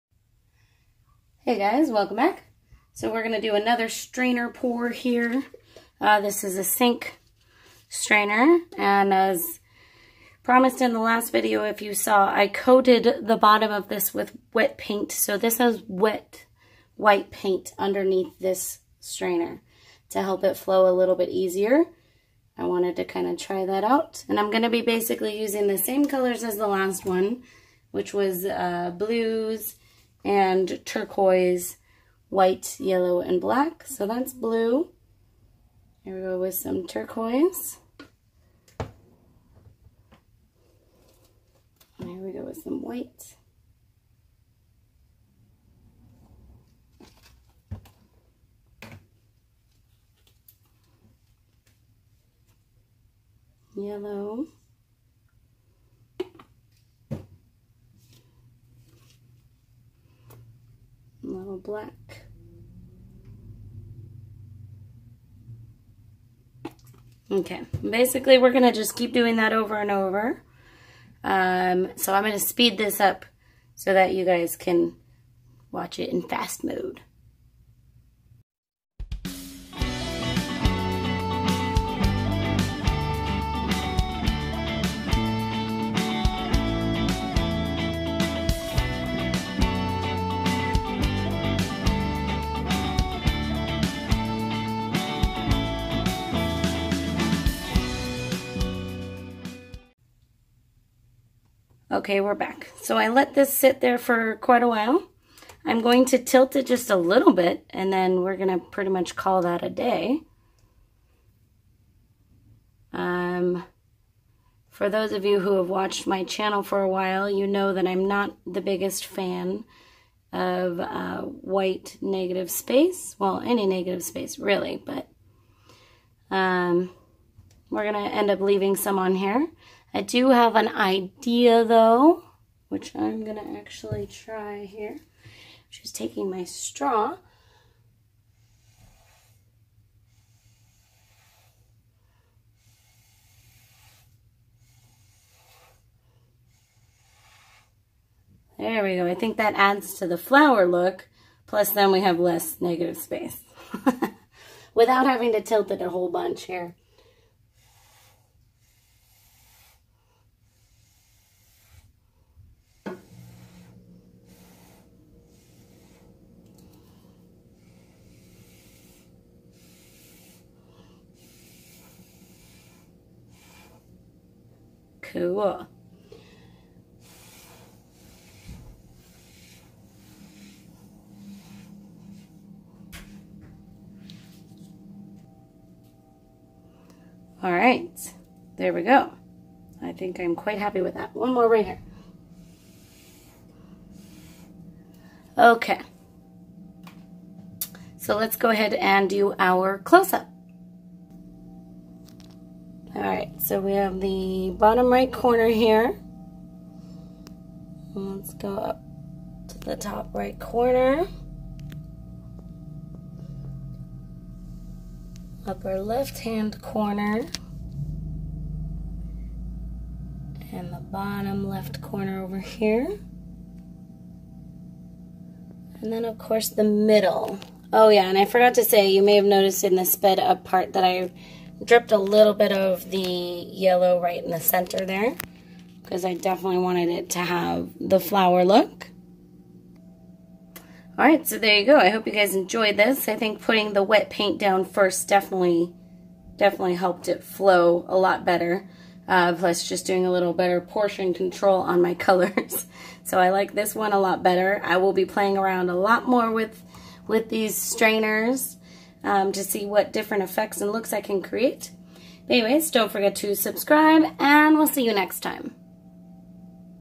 Acrylic Pour Through a Sink Strainer | Part 2
Acrylic pour through a sink strainer part 2! In this one I started out with a base of wet white paint (mixed the same as the rest of the paint) and also made my paints slightly more thin to help with the flow :) These sink strainers came from Amazon, you can check them out via purchase art supplies link below.

Materials used:

Artists Loft Flow Acrylics
Floetrol
Water
NO SILICONE

Mix ratio: 5 parts floetrol, 3 parts paint, 2 parts water